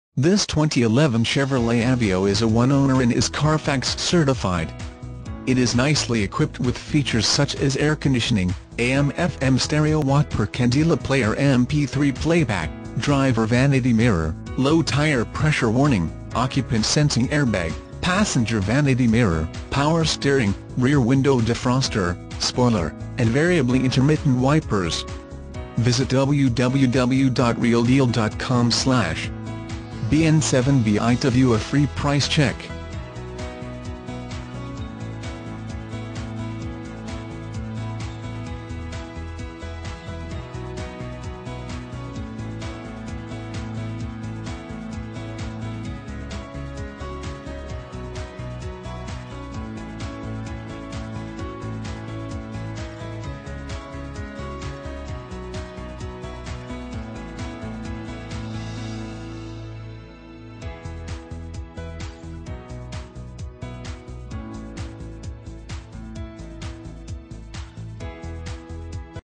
2011 CHEVROLET AVEO Cincinnati, OH 3-BB201668
2011 CHEVROLET AVEO Cincinnati, OH
Stock# 3-BB201668

Honda East
529 Ohio Pike

One Owner and is CARFAX Certified vehicle. Locally owned and Traded in. Fresh oil and Like New Tires! Tired of High gas prices? This little Aveo get 30+ MPG! to view a Free Price Check!